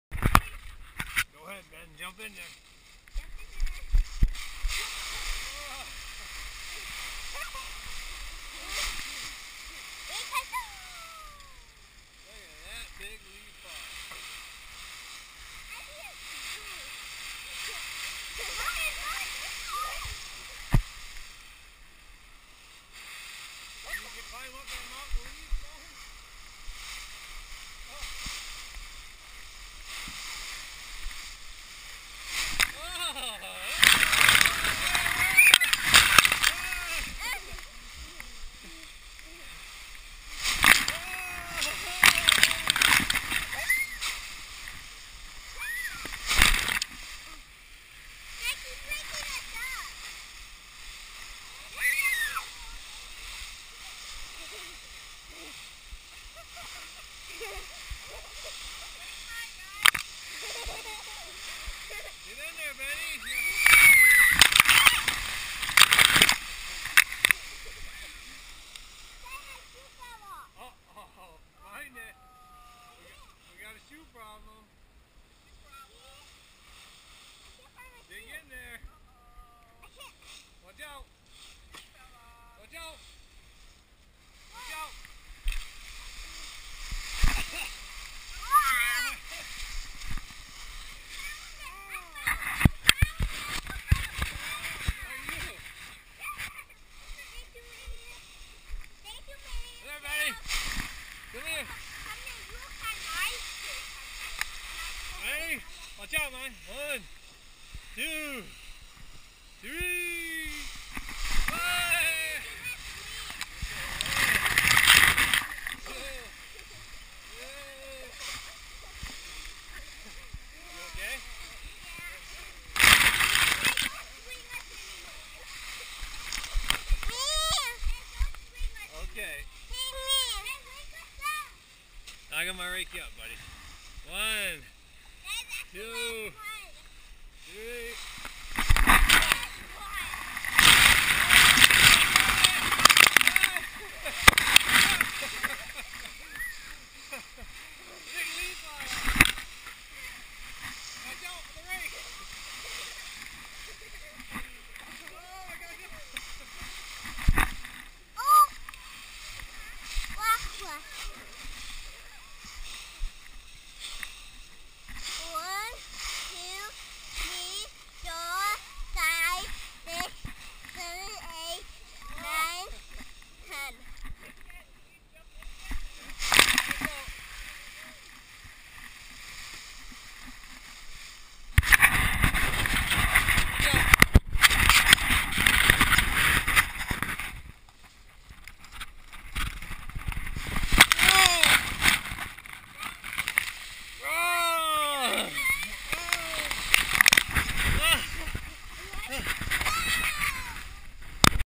benLeafPile
The huge, monster leaf pile and Benny's POV.